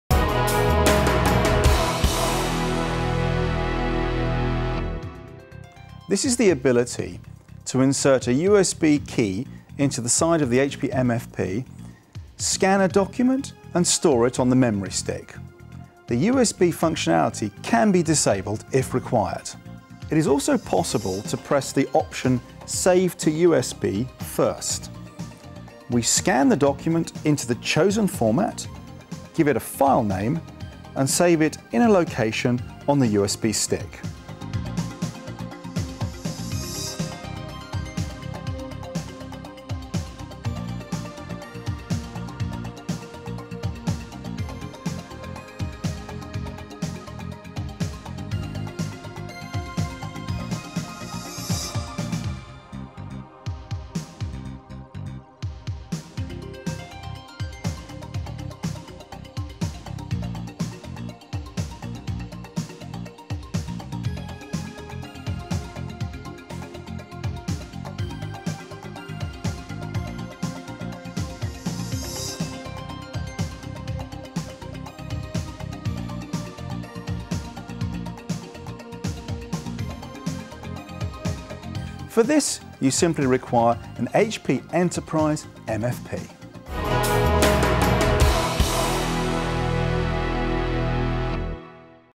Saving Documents at the HP MFP to USB
HP MFPs include USB slots that can be locked out or made available dependent upon customer requirement. When enabled it is possible to plug in a USB key into the HP MFP, search its directory structure, choose a file name and file format, and then scan a document straight to the USB key.